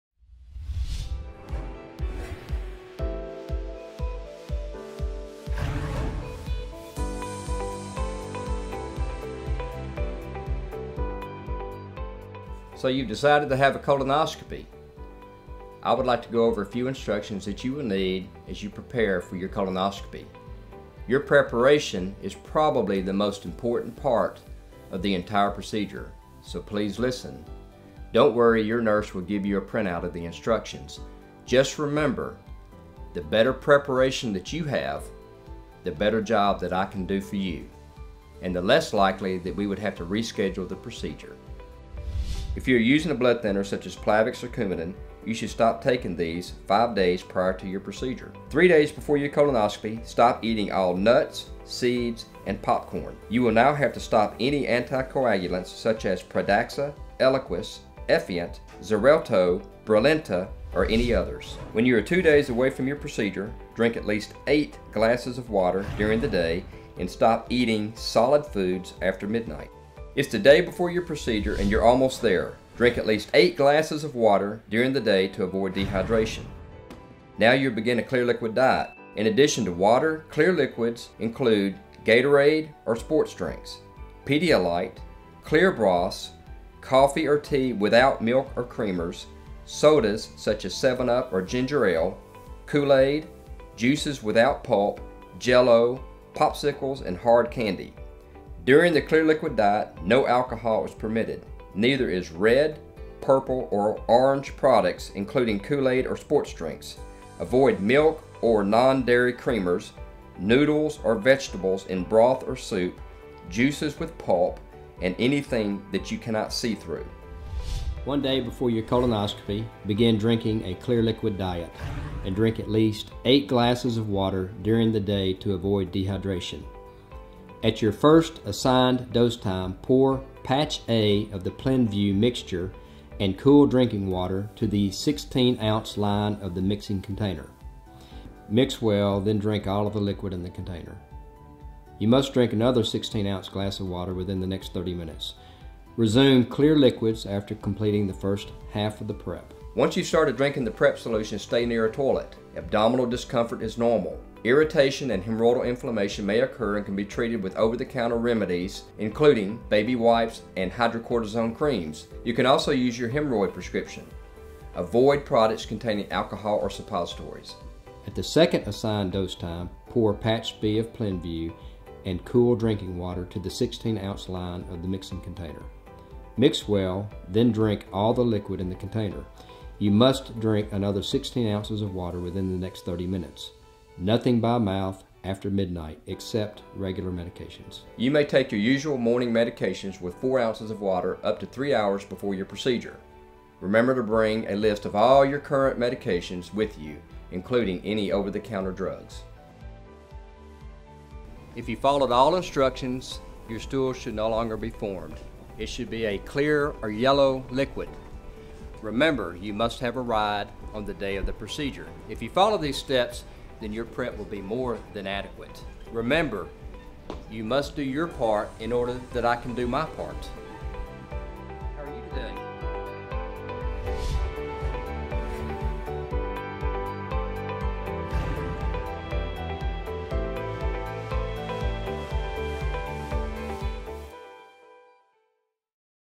Plenvu Colonoscopy Prep
Dr Morrissette explains proper colonoscopy preparation using Plenvu.